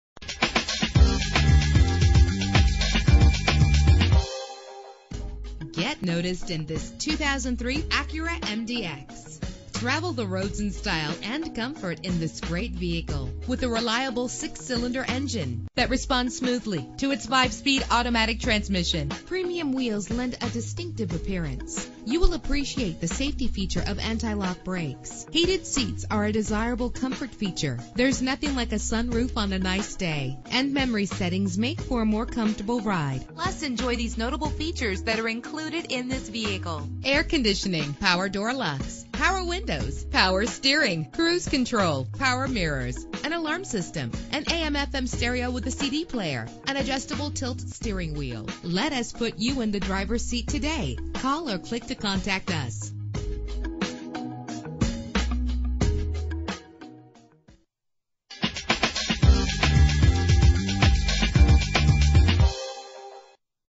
2003 Acura MDX - Dothan AL
Year: 2003
Make: Acura
Model: MDX
Trim: 3.5 Touring w/ RES
Engine: 3.5 liter 6 cylinder 24 valve
Transmission: 5-Speed Automatic
Color: Black
Mileage: 107430
Address:
4886 Montgomery Hwy